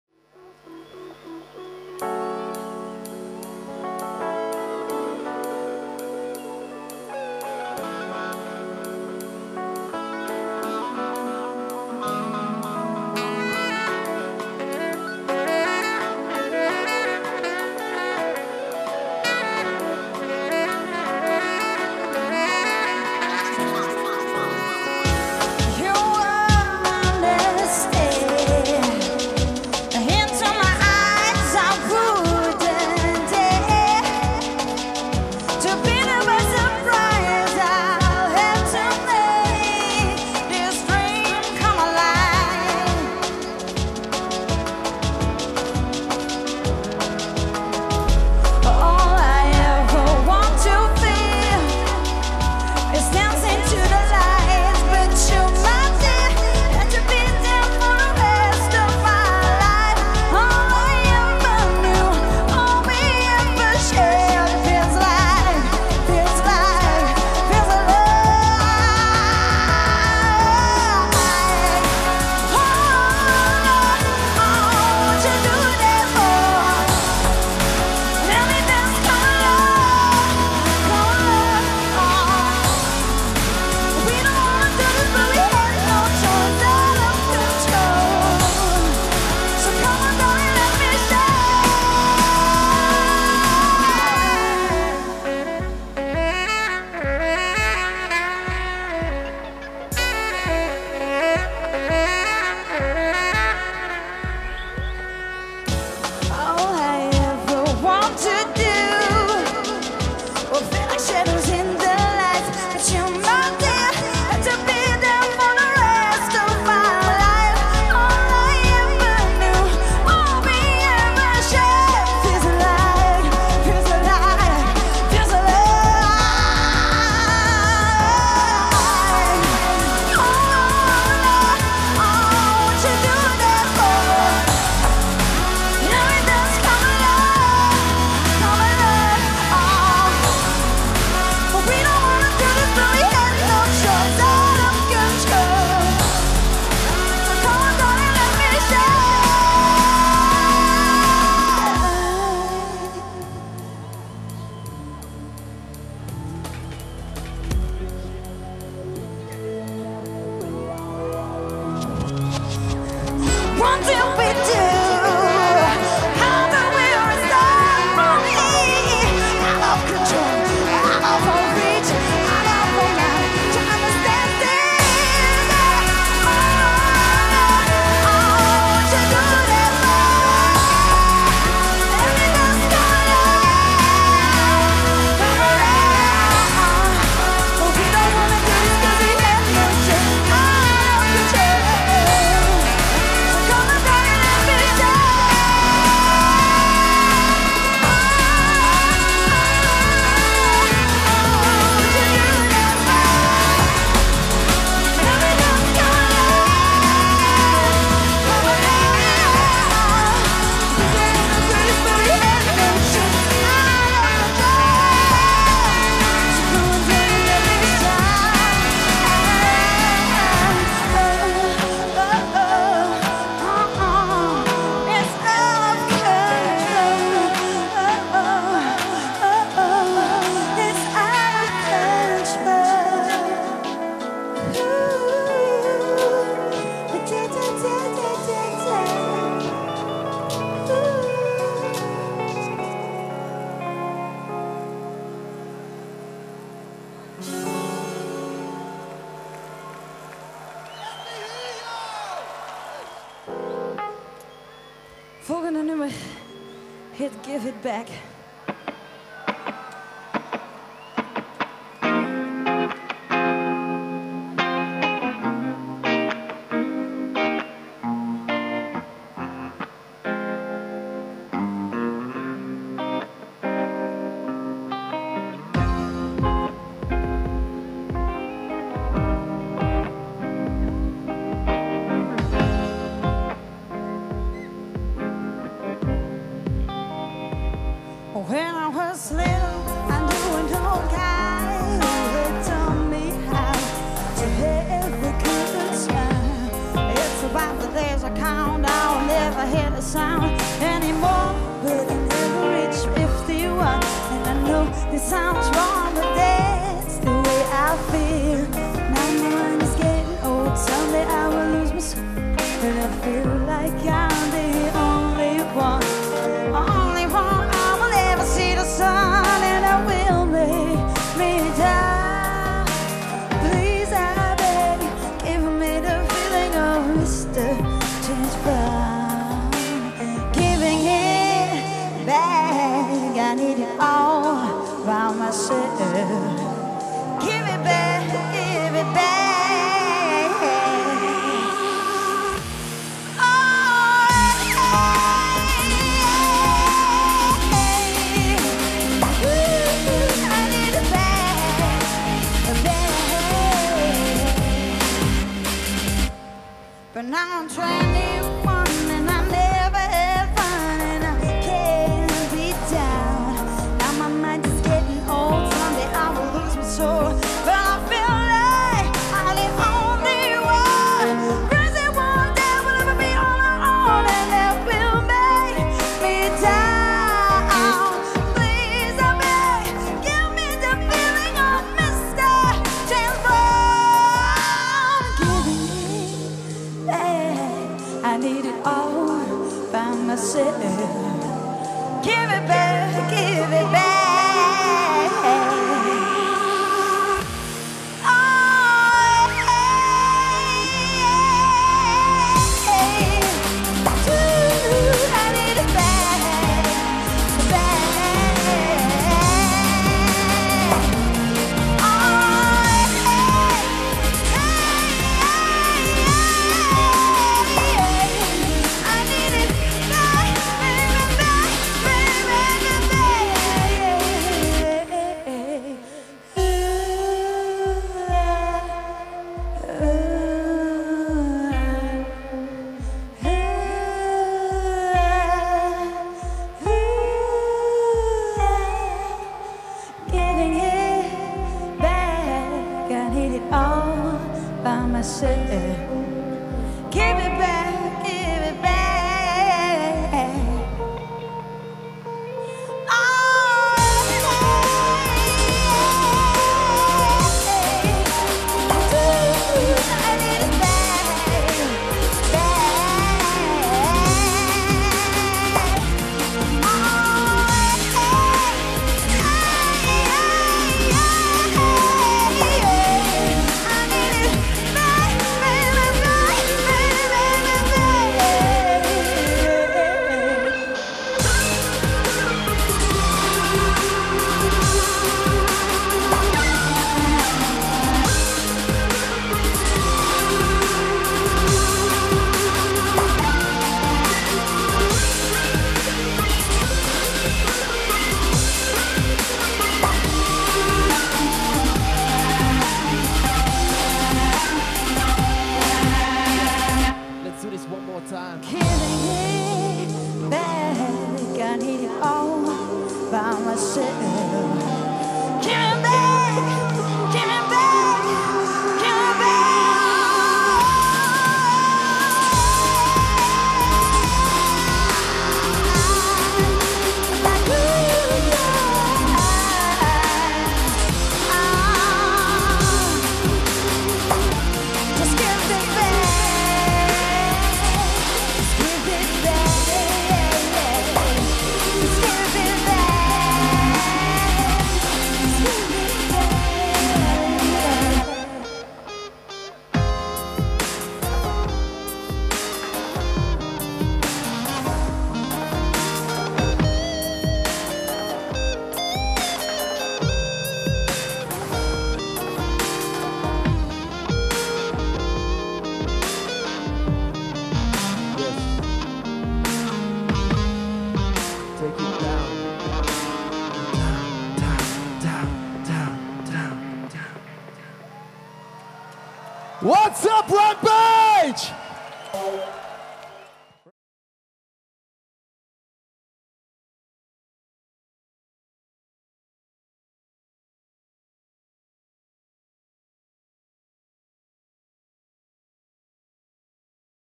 UKF vs RAMPAGE @ Lotto Arena Antwerp: AKS Live
UKF and RAMPAGE teamed up to bring you an epic show from the incredible Lotto Arena in Antwerp.15 acts including live performances rocked the audience through an unforgettable night.